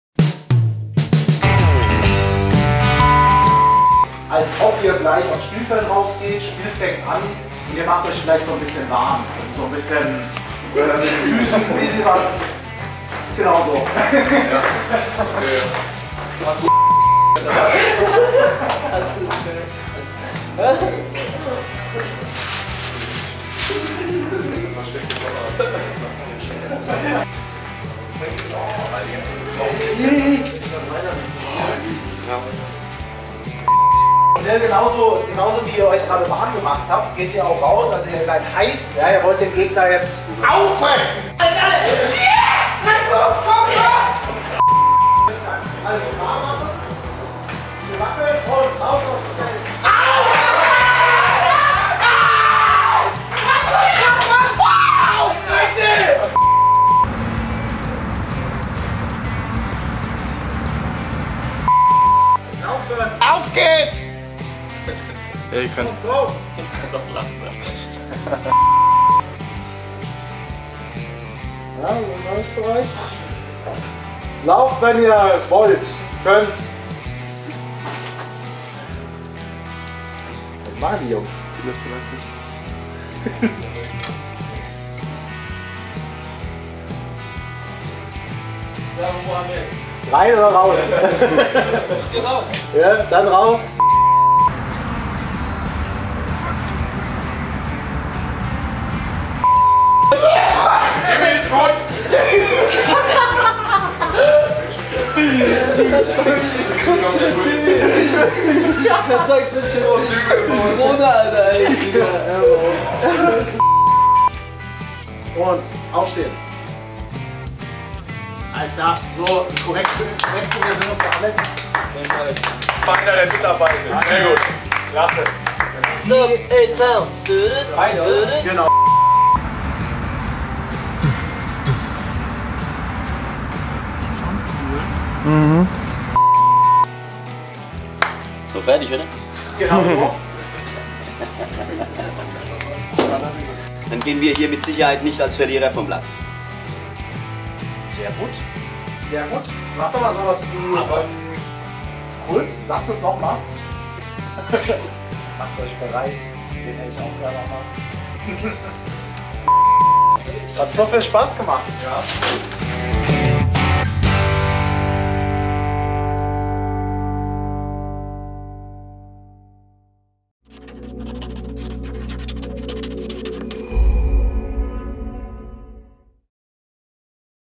FSV Imagefilm Videoproduktion Outtakes
Hierbei handelt es sich um Outtakes von unserer Videoproduktion des FSV Imagefilms.
Dieser Film wurde hauptsächlich in lokalen Kinos gezeigt.
Neben Spielern, Trainer und Geschäftsführer des FSV Frankfurt - findet man auch einige Mitarbeiter von You Name It vor der Kamera.
Das Video wurde hauptsächlich in der Volksbank Arena in Frankfurt produziert, wo der FSV Frankfurt heimisch ist.